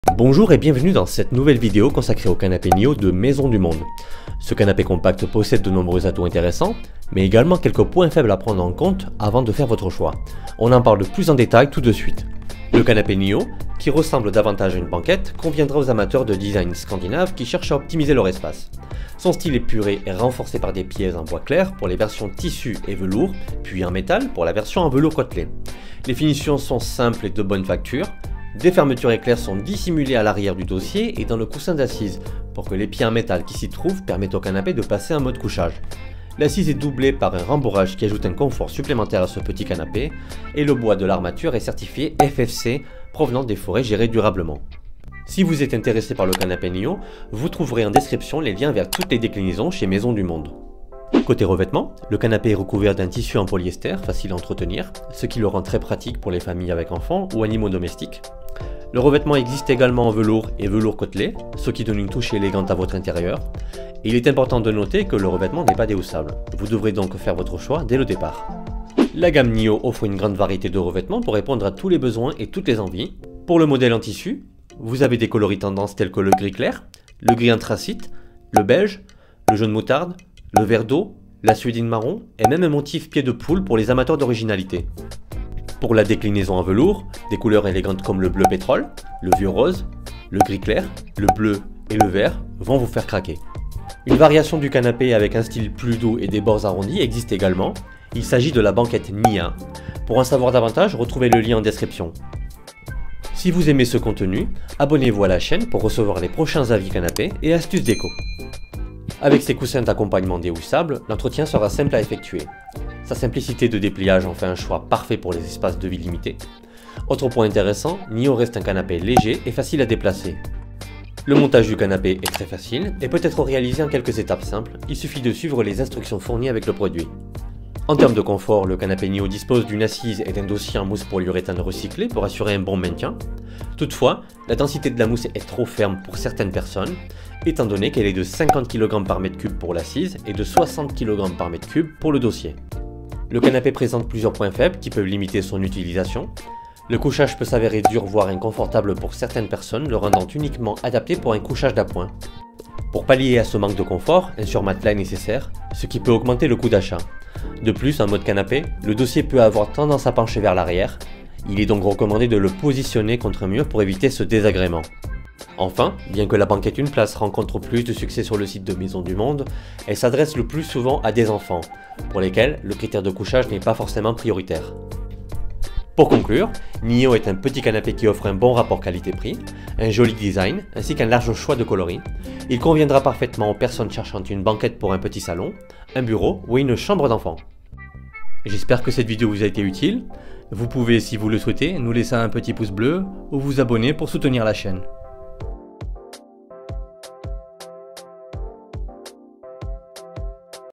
Avis Canapé NIO Maisons du Monde : Ce Qu'il Faut Savoir !
● A PROPOS DE NIO :

Le canapé Nio conviendra parfaitement aux passionnés de design scandinave cherchant à optimiser leur espace. Les versions en tissu et velours disposent de pieds en bois clair tandis que la version velours côtelé a des pieds en métal. Les fermetures éclairs cachées à l'arrière du dossier et dans le coussin d’assise permettent au canapé de se transformer en lit.

Le revêtement en polyester facile à entretenir est pratique pour les familles avec enfants ou animaux domestiques, tandis que le choix de revêtements en velours et velours côtelé ajoute une touche d'élégance. Les coussins d'accompagnement déhoussables rendent l'entretien facile. Cependant, la densité de la mousse peut être trop ferme pour certaines personnes et le couchage peut être inconfortable, ce qui en fait un choix idéal pour un couchage d'appoint seulement.

En conclusion, la banquette convertible Nio est un excellent choix pour les petits espaces tels que les petits salons, les bureaux ou les chambres d'enfant. Son design épuré, ses finitions de qualité et son choix de revêtements en font un choix pratique et élégant.

● SOMMAIRE :

00:00 • Intro
00:12 • Design
00:51 • Revêtements
01:09 • Déclinaisons
01:49 • Praticité
02:01 • Montage
02:08 • Confort
02:23 • Points faibles
02:56 • A qui s’adresse ce canapé ?
03:08 • Avis des canapés Maisons du Monde